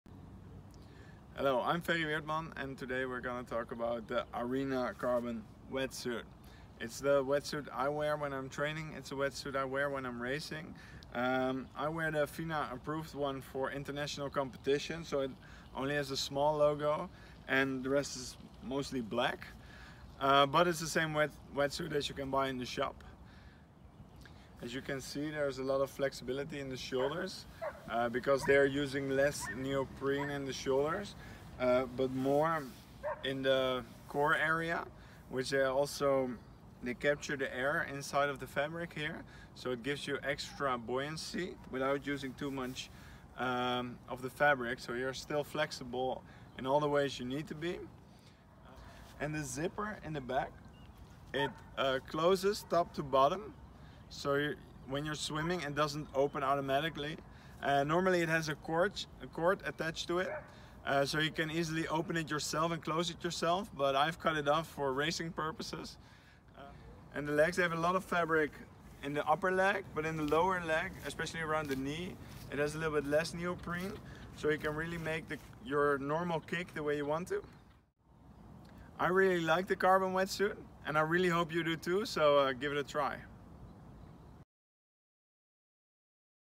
arena Carbon Wetsuit explained by Olympic Champion, Ferry Weertman
Olympic 10KM Open Water Champion, Ferry Weertman talks us through what he likes about our Carbon Wetsuit.

*Ferry is wearing the FINA Approved Suit for International Competition, so this variation holds fewer graphics than on the wetsuit sold

Featuring: Ferry Weertman